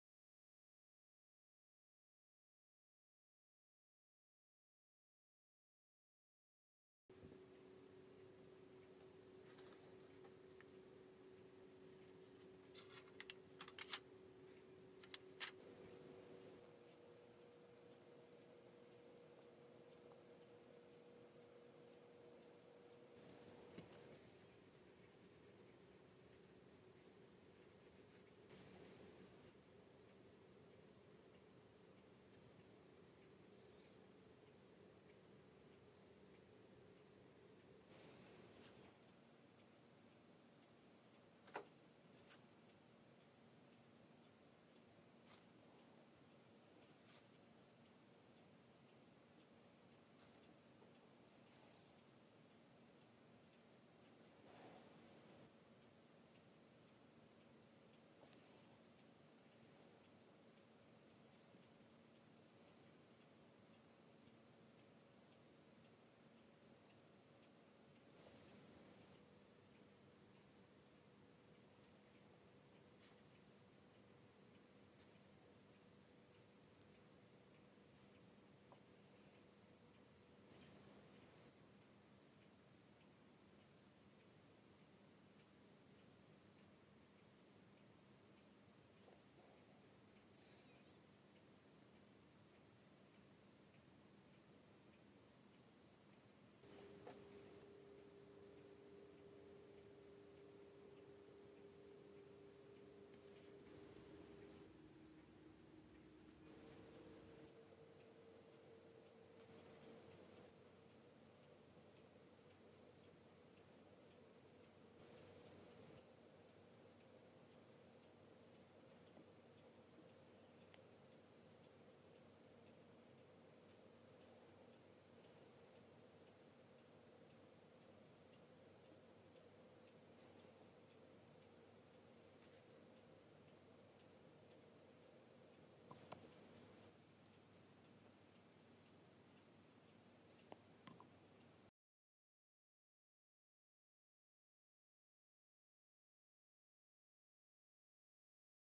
Memorial Day Nail Art - American Hero
This is a gradient red, white, and blue design with dog tags on the ring finger. Printed on the tags is a heart and the word Hero. I started this design with a white nail polish. All artwork was done using acrylic paints.

Nail Polish Used:
NK Really White

Get Connected:
Facebook (or search Nail Art By Kathleen)